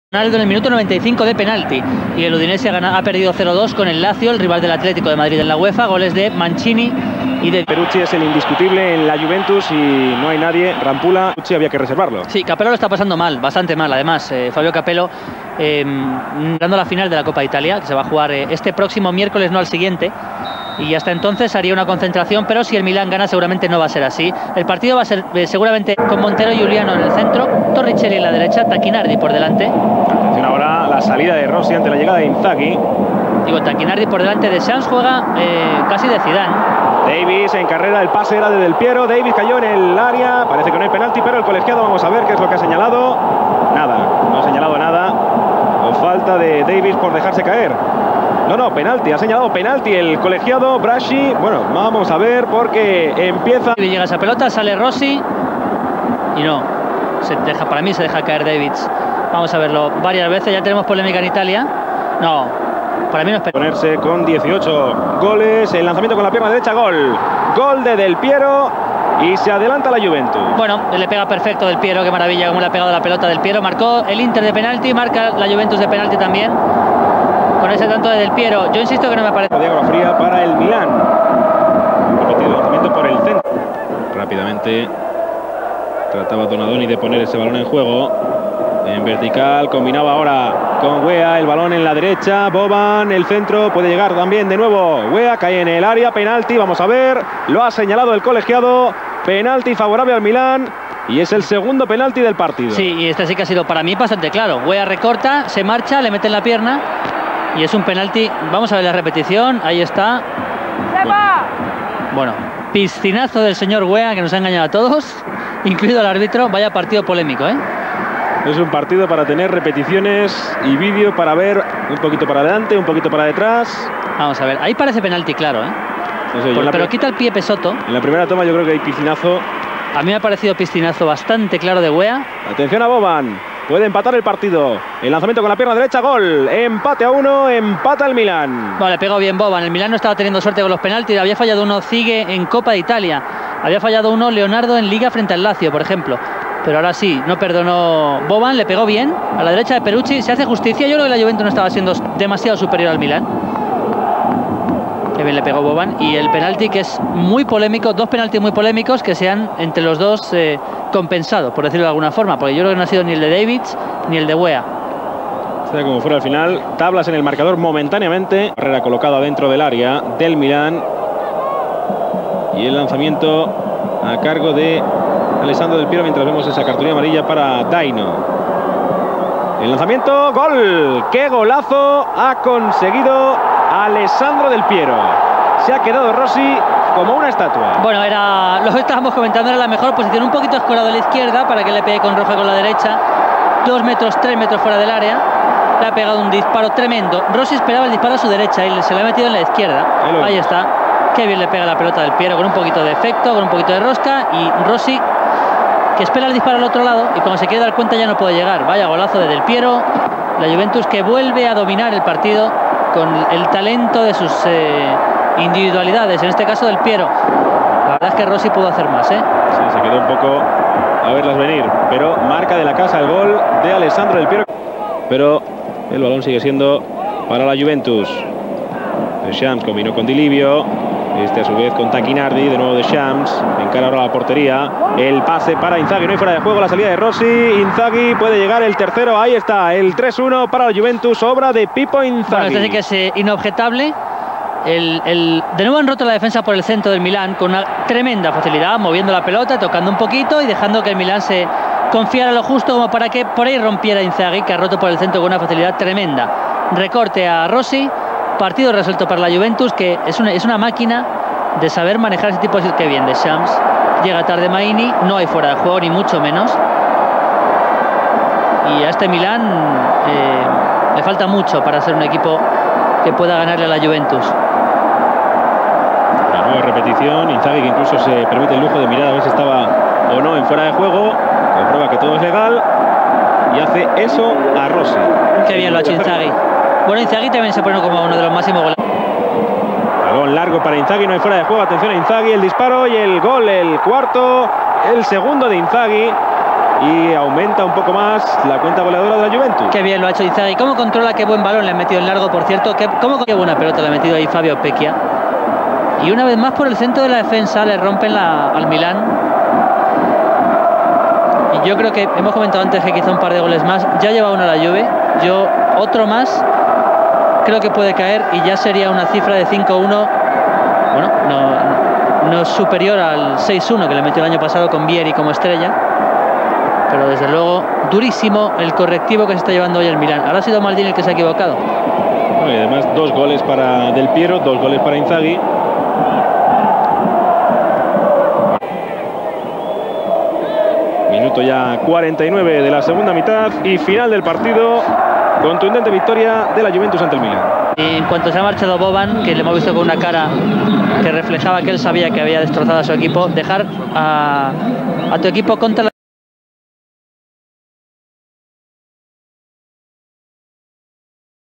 JUVENTUS 4-1 AC MILAN March 1997/98 SERIE A
2 GOALS FROM FILIPPO INZAGHI & DEL PIERRO SEALED A 4-1 EMPHATIC WIN AGAINST THEIR BIG RIVALS.